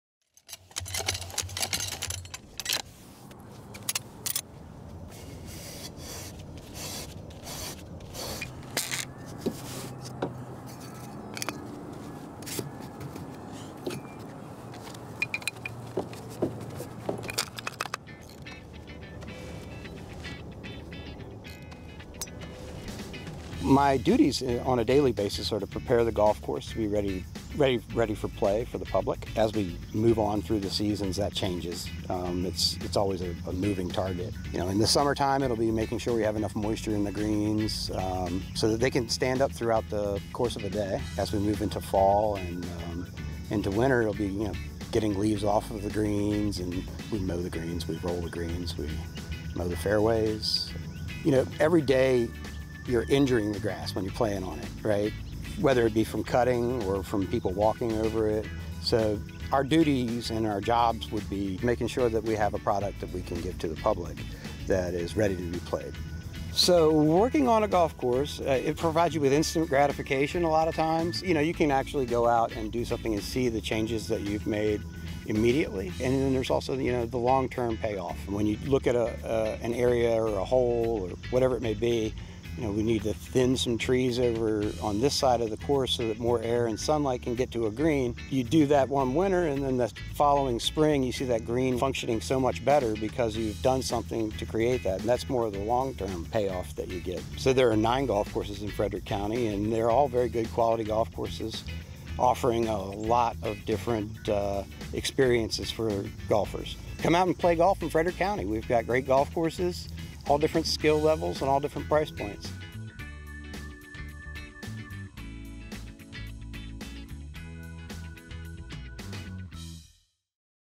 Destination Frederick County: Life of a Golf Course Superintendent
In this video, we had the opportunity to chat with Drew Metra, the Golf Course Superintendent at Whiskey Creek Golf Course. Join us as Drew takes us on a journey through his daily life, providing unique insights into what it's really like working at a golf course. Don't miss this exclusive behind-the-scenes look at the world of golf course management!